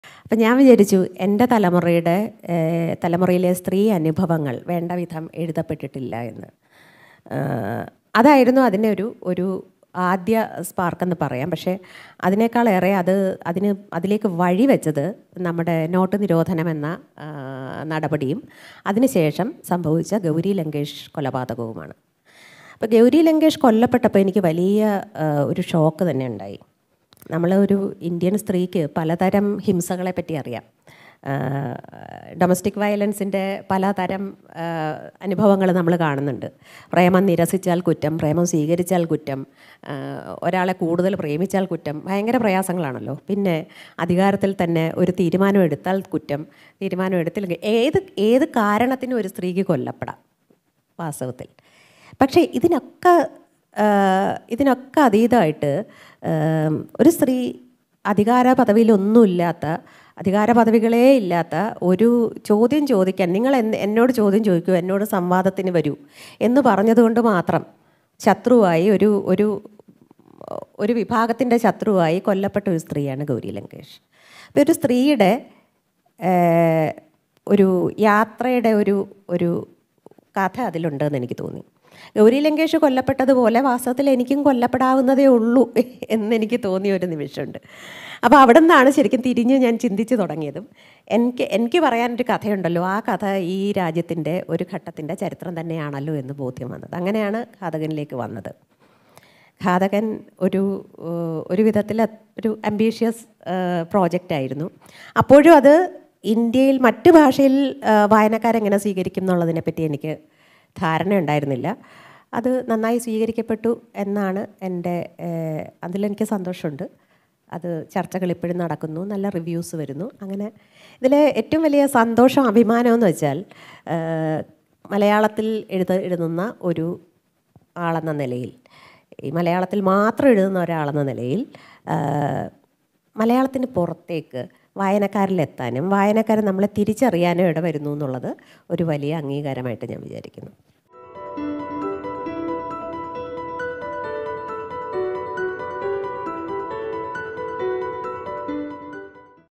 നിങ്ങൾ എന്നോട് സംവാദത്തിന് വരൂ എന്ന് പറഞ്ഞത് കൊണ്ട് മാത്രം കൊല്ലപ്പെട്ട സ്ത്രീയാണ് ഗൗരി ലങ്കേഷ്
നിങ്ങൾ എന്നോട് സംവാദത്തിന് വരൂ എന്ന് പറഞ്ഞത് കൊണ്ട് മാത്രം ഒരു വിഭാഗത്തിന്റെ ശത്രുവായി മാറി കൊല്ലപ്പെട്ട സ്ത്രീയാണ് ഗൗരി ലങ്കേഷ്.
ഇതുപോലെ ഞാനും കൊല്ലപ്പെടാവുന്നതേ ഉള്ളൂ എന്ന് തോന്നിയ ആ  നിമിഷം മുതലാണ് ഘാതകൻ എന്ന കഥ ഉണ്ടായത് : കെ. ആർ. മീര.

Gauri Lankesh | KR Meera | Sabha TV